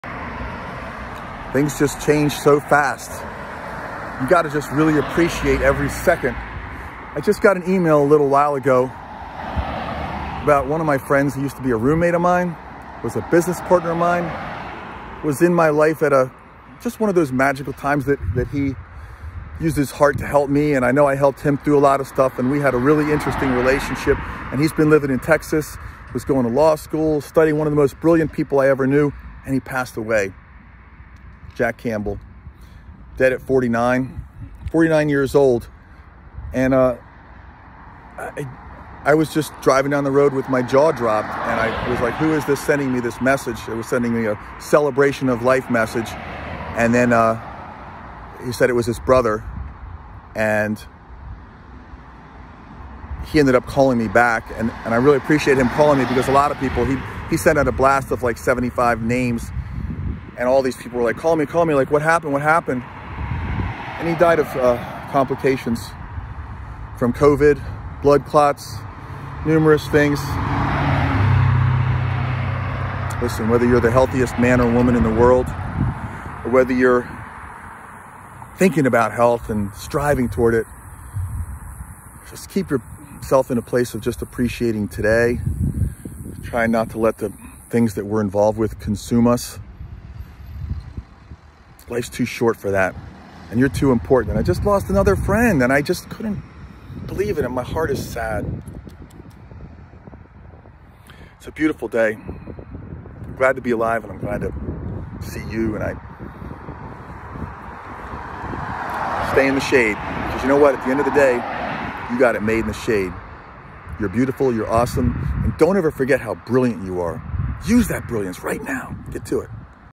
Enjoy every second of every day, my friends.
R.I.P. Jack Campbell. Gone at 49.

People come into our lives for reasons.

Often only for seasons.

If we can leave one another better than when we found one another,
Then our friendships will last forever...
And so will the memories.

It is my distinct pleasure to be swimming around this giant fish bowl with every single one of you.

Every person every second every time.

You matter.

To the business of living our best lives.

And EXpressing all that is in us.

In YOU. 🤜💥🤛

R.I.P. Jack.

Thank you for all that you brought into my world.
You affected the very course of it.
When it mattered the most.

All love.

Tim